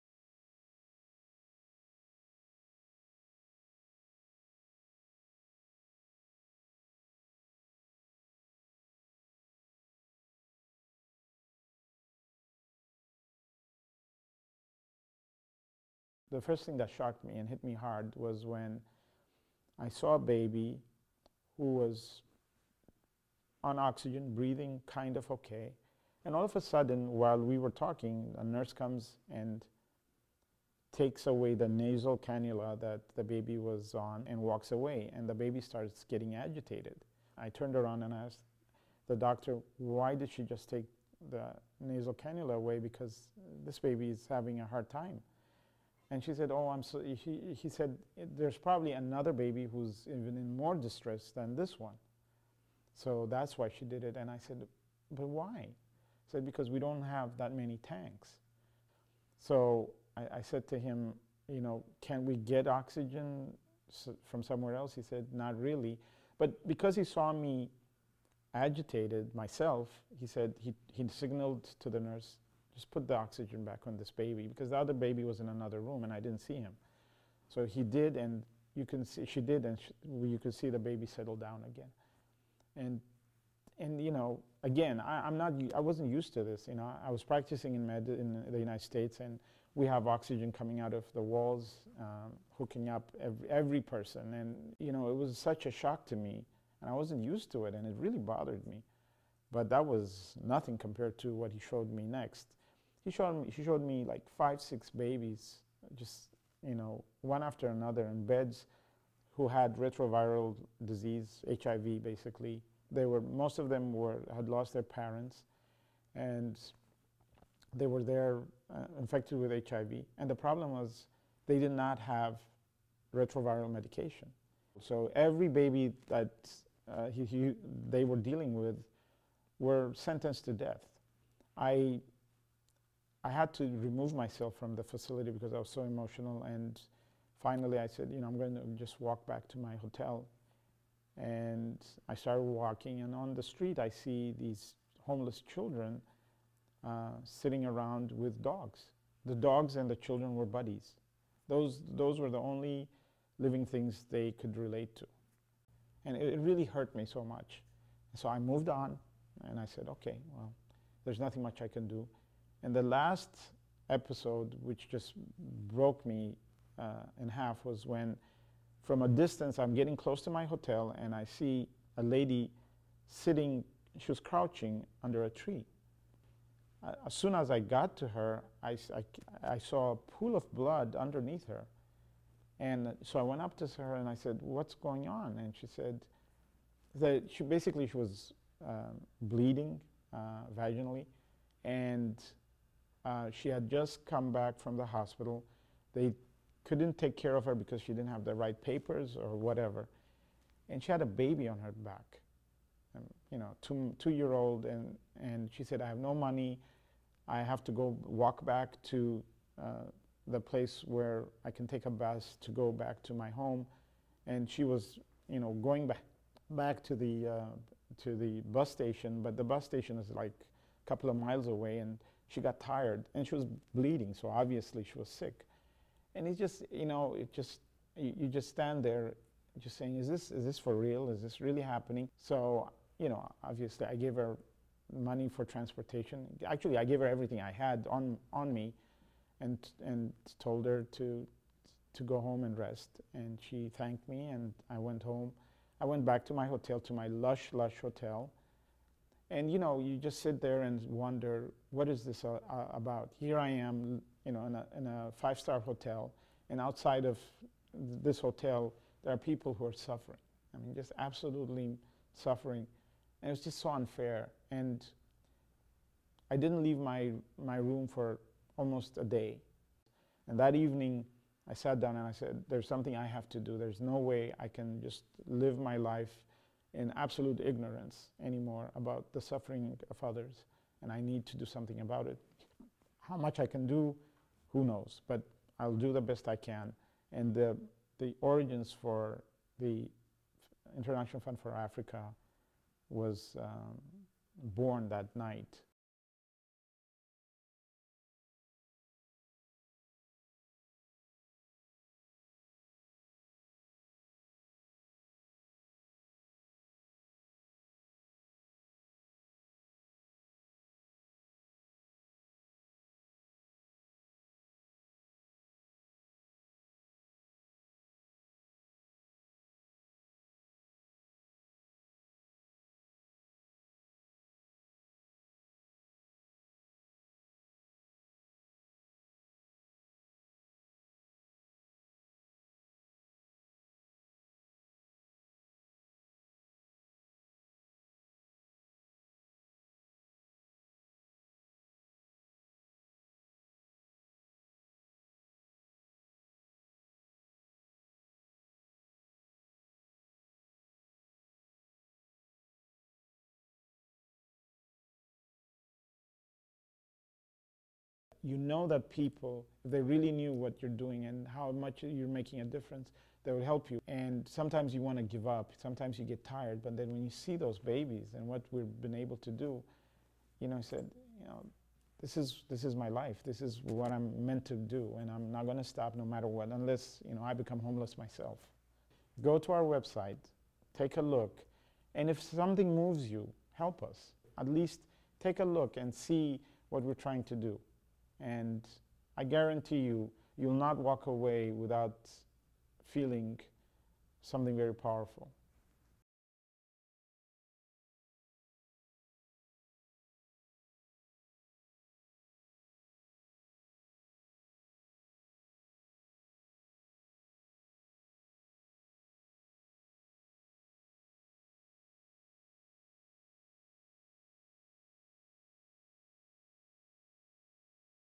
International Fund for Africa Mission Statement
Dr. Anteneh Roba talks about what prompted him to start International Fund for Africa, and the objectives of this charity.